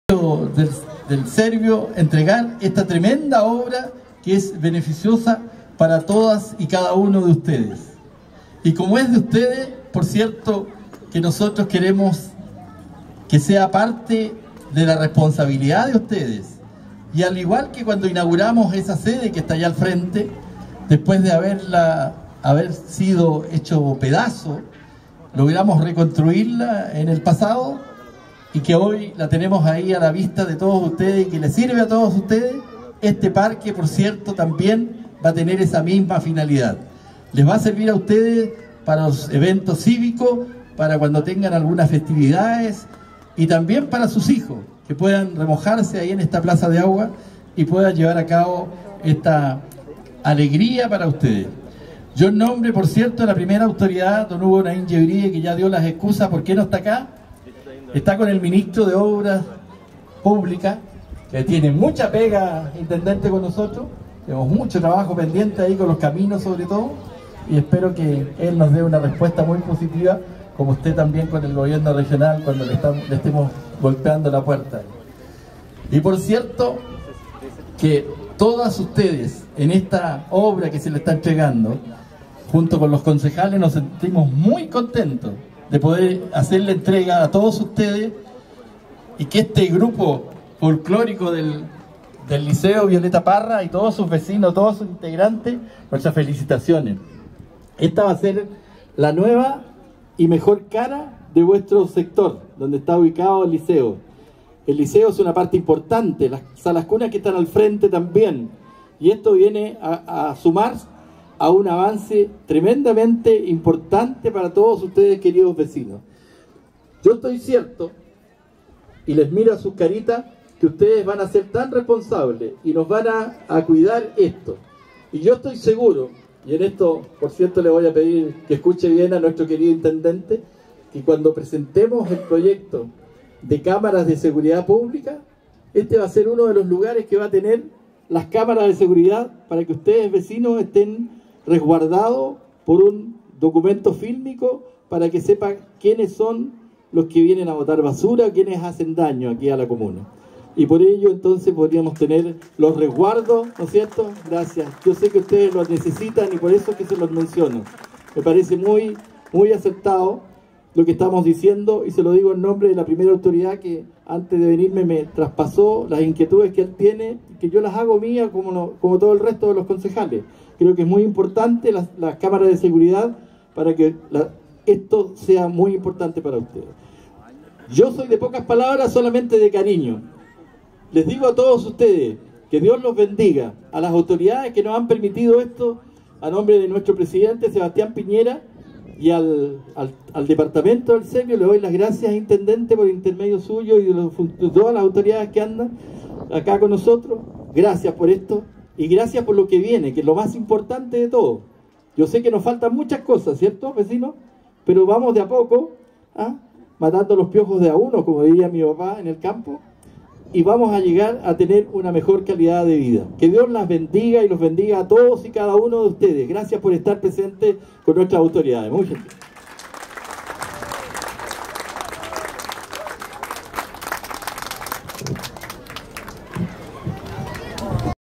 Inauguración Parque Central Pobl. Once de Septiembre, San Carlos - Vídeo 2
5 sept. 2018.- Palabras de Alcalde Protocolar, Sr. Pedro Méndez.
Fotos en Fan Page San Carlos Noticias.